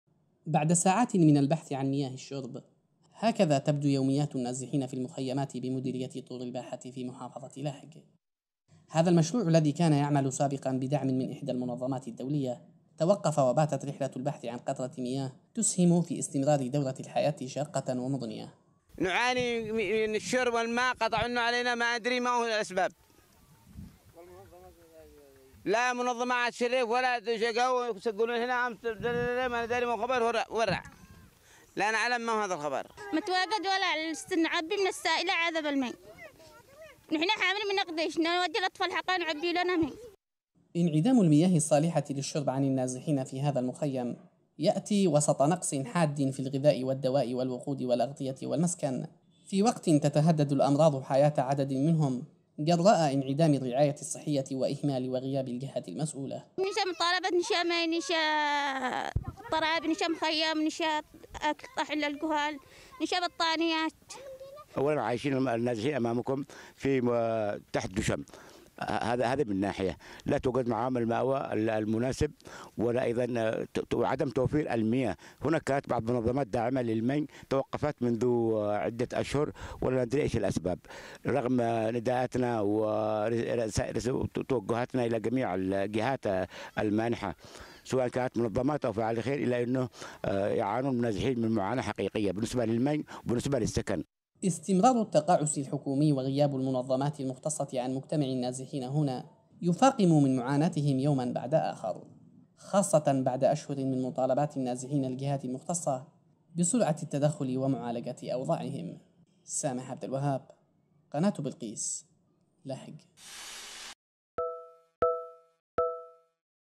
تردي الأوضاع المعيشية للنازحين في لحج جراء انعدام مياه الشرب ونقص الغذاء | تقرير: سامح عبد الوهاب
يعيش النازحون في محافظة لحج وضعا مترديا جراء انعدام المياه ونقص الغذاء والمأوى .
حيث انعدمت مياه الشرب في المخيمات بمديرية طور الباحة غرب المحافظة نتيجة توقف العمل بأحد مشاريع المياه المخصصة لهم...